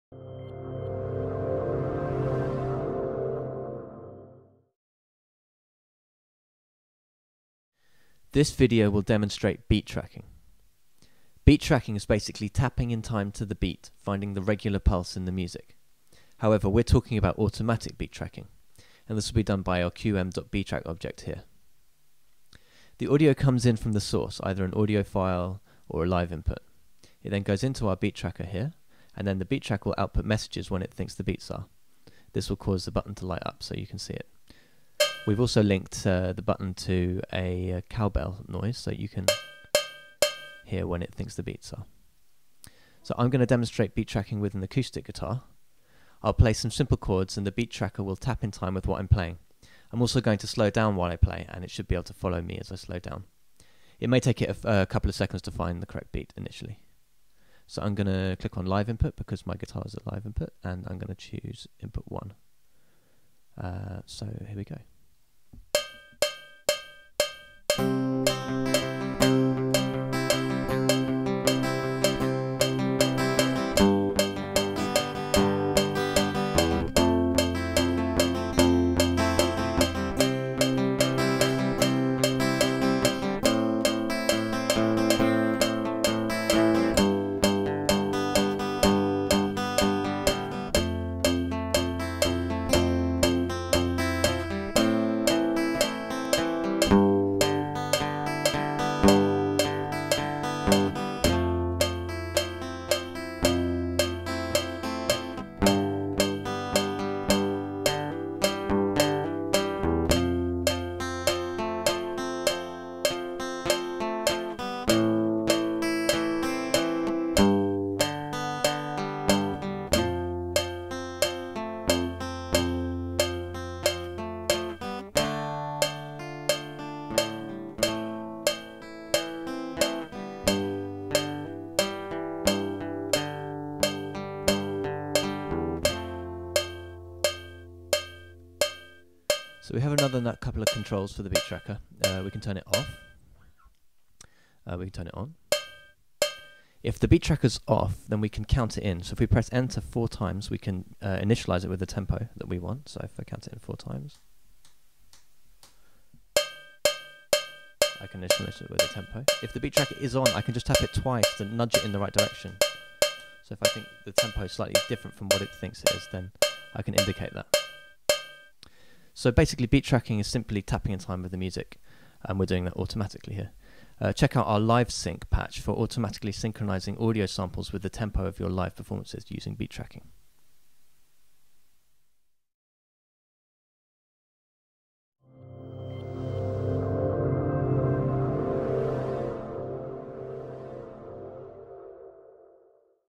Beat tracking using qm.btrack~ - Live Music Lab
Beat tracking using qm.btrack~. Bring a new dimension to your live music performance.

This series of videos introduces brand new digital music technology developed by Queen Mary, University of London's Live Music Lab which automatically synchronises your pre-recorded samples to the exact tempo of live music. This tutorial series shows how, using Harmonic Analysis, Beat Tracking and Colourmapping components, we can use LiveSync technology to do in a second what would otherwise take us mere humans hours to perfect.